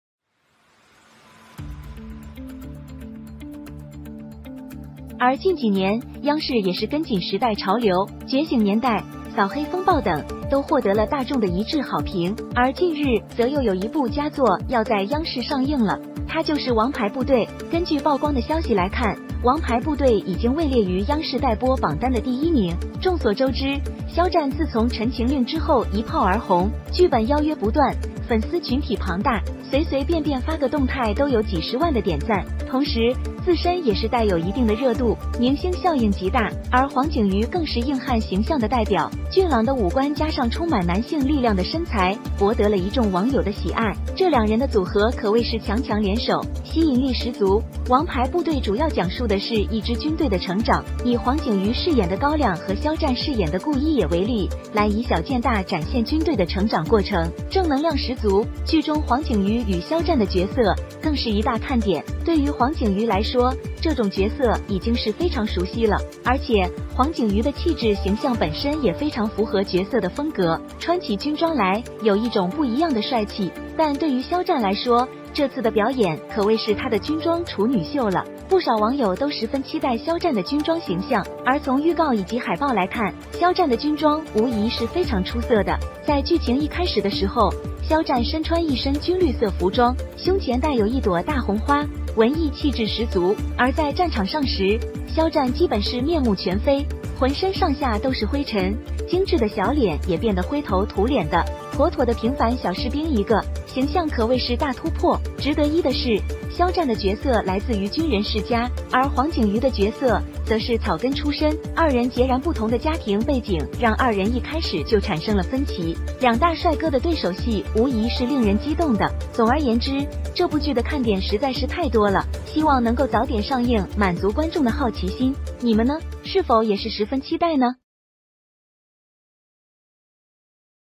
It was broadcast on CCTV, Xiao Zhan and Huang Jingyu joined forces, netizens: You can't watch
The TV drama circle basically takes CCTV as the wind vane, and also has a "CCTV production must be fine" call, basically can be broadcast in CCTV drama ratings will not be too low. In recent years, CCTV is also keeping up with the trend of the times, "Awakening Age", "Black Storm" and so on have won the public's unanimous praise, and recently there is another masterpiece to be screened in CCTV, it is "Ace Force". According to the news, "Ace Force" has been ranked first on the CCTV list. The probability of broadcast is very high, and recently "Ace Force" has also obtained the license of distribution, it can be said to be a double happiness, and nothing is happier than the fans of the main actors in the drama. The surprise is that the main actors of this drama are Xiao Zhan and Huang Jingyu. As we all know, since the "Chen Eros" after a hit, script offers constantly, the fan base is huge, casually send a dynamic have hundreds of thousands of likes, but also with a certain degree of heat itself, the star effect is great. The two of them can be described as a strong team, attractive. The main story of "The Ace Force" is the growth of an army, with Huang Jingyu as Gao Liang and Xiao Zhan as Gu Yino as examples to show the growth process of the army in a small way, full of positive energy. The role of Huang Jingyu and Xiao Zhan is a major point of interest. For Huang Jingyu this role is already very familiar, and Huang Jingyu's temperament image itself is also very consistent with the style of the role, wearing a military uniform to have a different kind of handsome. But for Xiao Zhan this performance can be his military debut, many netizens are very much looking forward to Xiao Zhan's military image. And from the previews and posters, Xiao Zhan's military uniform is undoubtedly very good. At the beginning of the episode, Xiao Zhan wore a military green clothing, with a large red flower on the chest, literary temperament; and in the battlefield, Xiao Zhan is basically a face, all over the body is dust, delicate face also became gray, properly ordinary small soldiers a, image can be described as a big breakthrough. It is worthwhile to note that Xiao Zhi's role comes from a military family, while Huang Jingyu's role is grassroots origin, their very different family backgrounds make them diverge at the beginning, the two handsome rivalry is undoubtedly exciting. The other major point is the production level of this drama. First of all, the production team of this drama belongs to the top level, but also produced a number of military theme works, this area of proficiency is very high, the film effect naturally do not worry. Secondly, every picture of this drama is very real, the plot structure is also very reasonable, compared to some crude drama is not a little bit stronger. Because of the military theme, some of the necessary fight scenes are indispensable, tanks, submachine guns and so on are in turn on the line, enough to see the sincerity of the crew. In the end, the two main actors, Xiao Zhi Huang Jing Yu, the rest of the supporting cast are also strong actors. For example, one of the few female roles played by Zhong Chuxi, as a newly emerging flower acting skills are also noteworthy. The actor Li Youbin is the ceiling of the military drama, once the "Bright Sword" let many people remember the vivid Li Yunlong, will Li Yunlong's glorious image is shown to the full. And He Zhengjun is a national-level actor, starred in a number of military-themed film and television works, won a number of awards, such a configuration who do not feel too top? All in all, this drama is too much to see, I hope to be released early to meet the curiosity of the audience. What about you? Are you also very much looking forward to it?